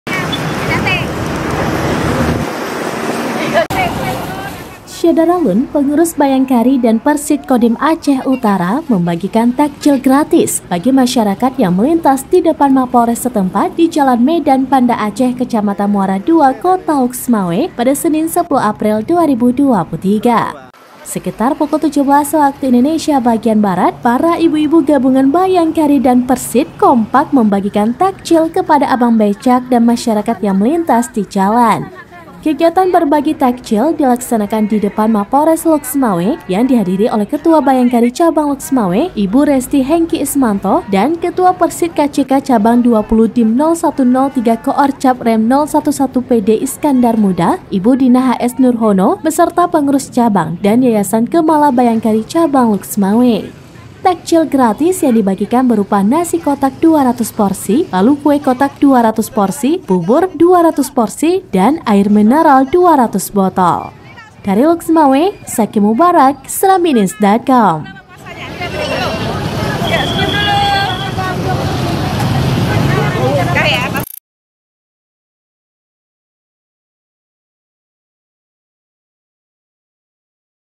Ibu Bhayangkari dan Persit Bagi-bagi Takjil Gratis di Depan Mapolres Lhokseumawe
Laporan Zaki Mubarak | Lhokseumawe
SERAMBINEWS.COM, -  Pengurus Bhayangkari dan Persit Kodim Aceh Utara membagikan takjil gratis bagi masyarakat yang melintas di Depan Mapolres setempat di Jalan Medan-Banda Aceh, Kecamatan Muara Dua, Kota Lhokseumawe, Senin (10/4/2023).

Sekitar pukul 17.00 WIB para ibu-ibu gabungan Bhayangkari dan Persit kompak membagikan takjil kepada abang becak dan masyarakat yang melintas di jalan.

Kegiatan berbagi takjil dilaksanakan di depan Mapolres Lhokseumawe yang dihadiri oleh Ketua Bhayangkari Cabang Lhokseumawe Ibu Resti Henki Ismanto dan Ketua Persit KCK Cab XX Dim 0103 Koorcab Rem 011 PD Iskandar Muda Ibu Dina HS Nurhono beserta pengurus cabang dan yayasan Keumala Bhayangkari Cabang Lhokseumawe.

Takjil gratis yabg dibagikan berupa nasi kotak 200 porsi, lalu kue kotak 200 porsi, bubur 200 porsi dan air mineral 200 botol